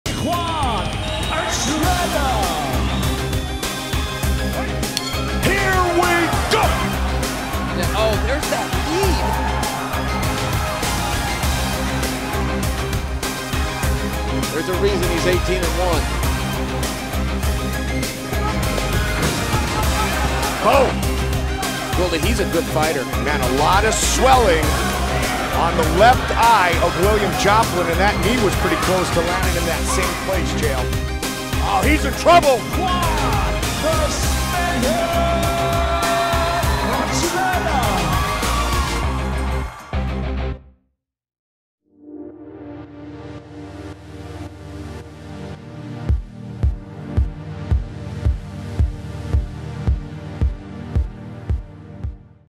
Bellator 201: Juan Archuleta Highlights
Take a look at Juan Archuleta's highlights from Bellator 195! And don't miss his upcoming fight against Robbie Peralta at Bellator201!

Bellator201 | FRIDAY, June 29th, 2018 | On Paramount Network at 9/8c